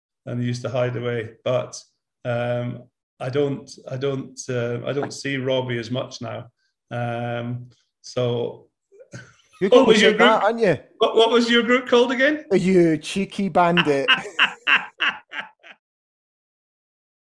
St Mirren v Aberdeen Preview Show tease.
On Saturday I will post a pre-recorded preview show as I look ahead to our vital game against St Mirren. Come and join the festive fun as former goalkeeper Bryan Gunn and I work our way through some quick-fire questions.